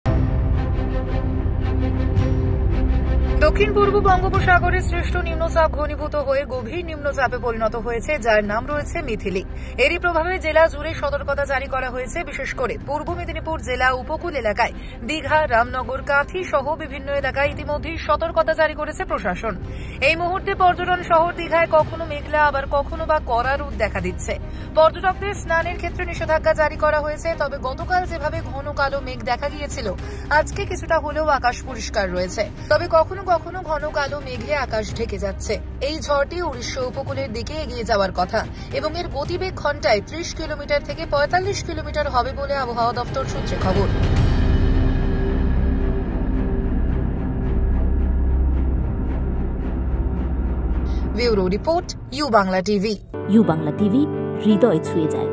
Kolkata|দক্ষিণ-পূর্ব বঙ্গোপসাগরের সৃষ্ট নিম্নচাপ ঘনীভূত হয়ে গভীর নিম্নচাপে পরিণত হয়েছে |U BanglaTV
দক্ষিণ-পূর্ব বঙ্গোপসাগরের সৃষ্ট নিম্নচাপ ঘনীভূত হয়ে গভীর নিম্নচাপে পরিণত হয়েছে যার নাম রয়েছে মিথিলি, এরই প্রভাবে জেলা জুড়ে সতর্কতা জারি করা হয়েছে বিশেষ করে পূর্ব মেদিনীপুর জেলা উপকূল এলাকায় দীঘা রামনগর কাঁথি সহ বিভিন্ন এলাকায় ইতিমধ্যে সতর্কতা জারি করেছে প্রশাসন। এই মুহূর্তে পর্যটন শহর দীঘায় কখনো মেঘলা আবার কখনো বা কড়া রোদ দেখা দিচ্ছে। পর্যটকদের স্নানের ক্ষেত্রে নিষেধাজ্ঞা জারি করা হয়েছে ‌ তবে গতকাল যেভাবে ঘন কালো মেঘ দেখা গেছিল আজকে কিছুটা হলেও আকাশ পরিষ্কার রয়েছে ‌ তবে কখনো কখনো ঘন কালো মেঘ আকাশ ঢেকে যাচ্ছে। এই ঝড়টি উড়িষ্যা উপকূলের দিকে এগিয়ে যাওয়ার কথা এবং এর গতিবেগ ঘন্টায় ৩০ কিলোমিটার থেকে ৪৫ কিলোমিটার হবে বলে আবহাওয়া দপ্তর সূত্রে খবর।